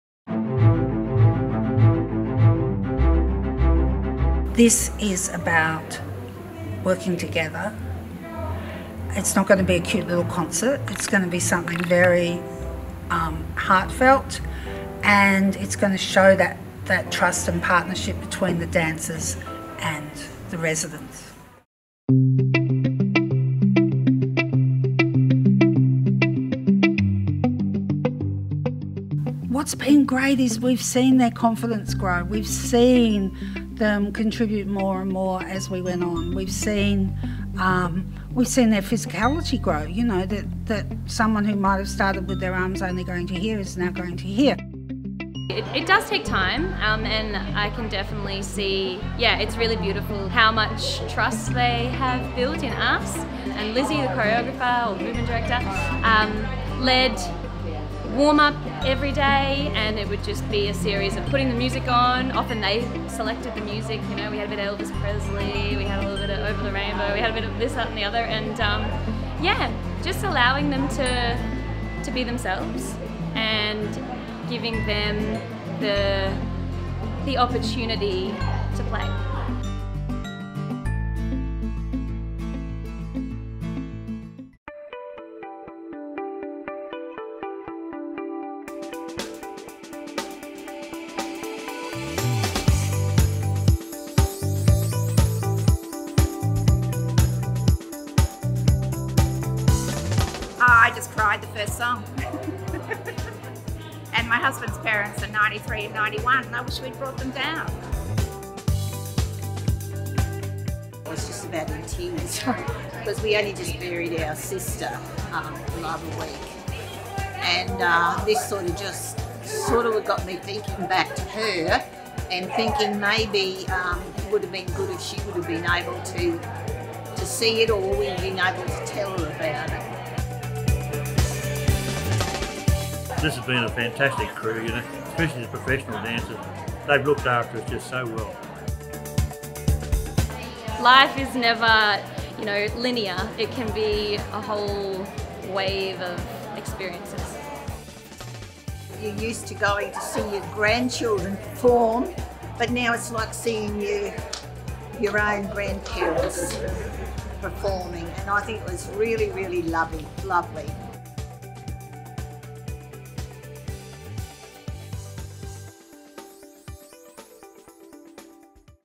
The Making of If Only I Could…
If Only I Could… spotlights an alliance between a group of elderly aged care residents and professional contemporary dance artists in a collaboration between Lutheran Services, QPAC and director Angela Chaplin.

These amazing elders take to the stage with grace and courage, a great sense of adventure, and a greater sense of humour to share a whimsical, magical, heartfelt performance that will have you smiling from ear to ear, and maybe shedding a tear. Projected imagery provides a window into big lives and great times, where wheelie walkers will fly and cowboys will round you up.

By forming friendships, sharing stories, making new discoveries, and testing the abilities of their bodies and minds the aged care residents and professional dance artists have made a show quite unlike anything you’ve ever seen.

Humorous, irreverent but chiefly a complex waltz between generations, let this production open your heart and move your spirit.